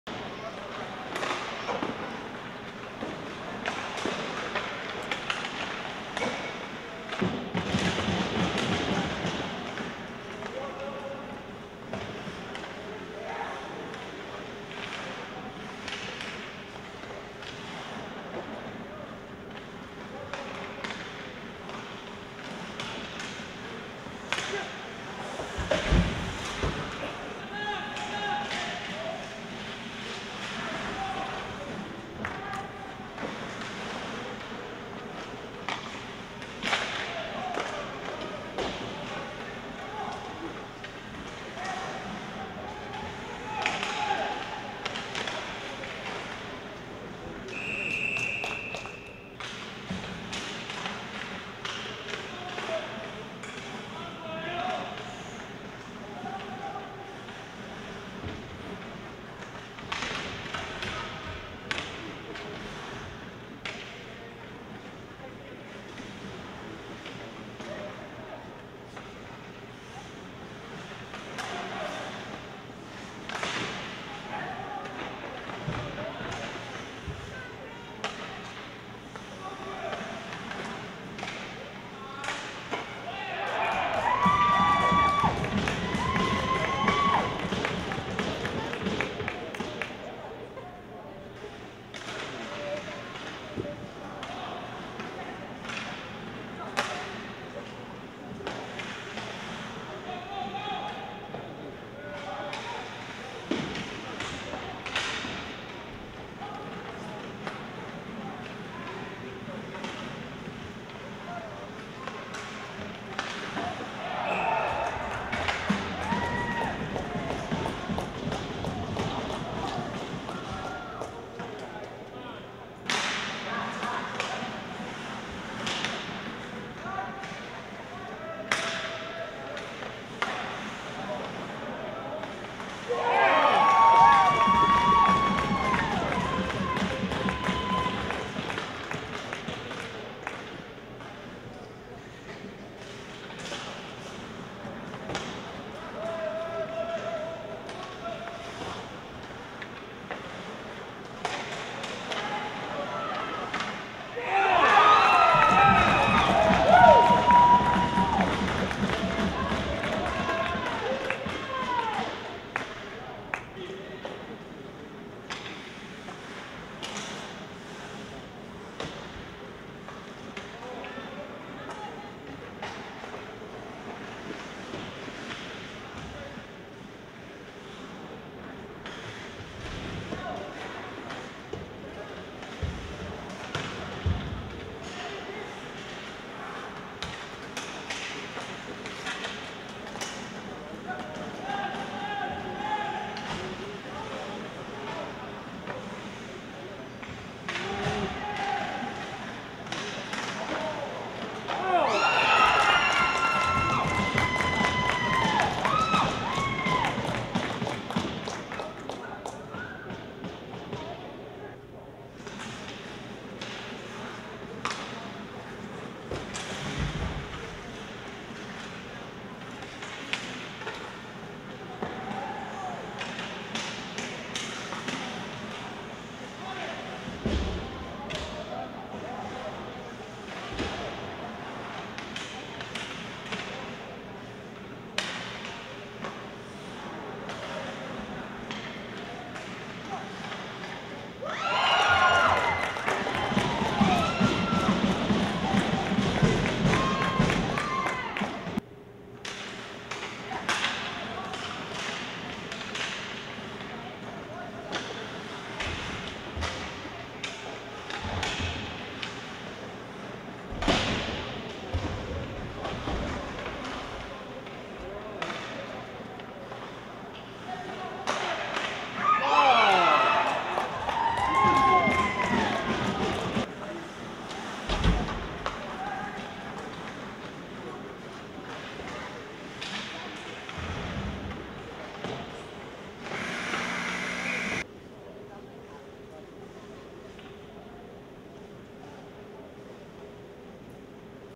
Jimmy Johns, MI v Cleveland Barons, OH // U16 Tournament Game
Watch Jimmy Johns (MI) take on the Cleveland Barons (OH) in this 2016 Bauer World Invite U16 Tournament Game.